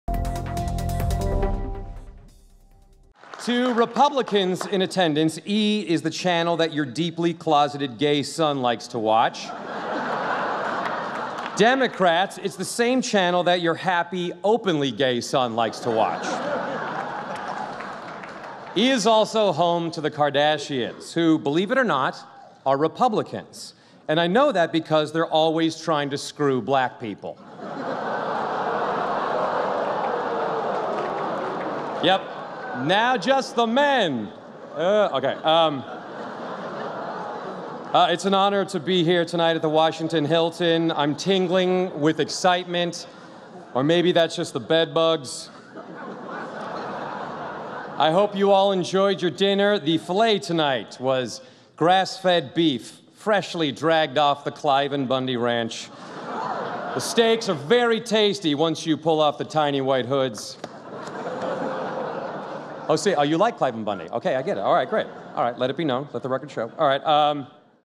Joel McHale on Gays, Kardashians, Cliven Bundy
White House Correspondents' Dinner headliner Joel McHale meets some resistance from the crowd with a set of jokes about politicians' gay sons, the Kardashians, and Nevada rancher Cliven Bundy.